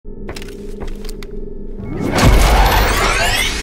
AAC24 Day 12: Battle Shapers
Day 12 of the AAC24 with some punchy sci-fi sounds for a clip of the game "Battle Shapers". Make sure to check out the game on Steam!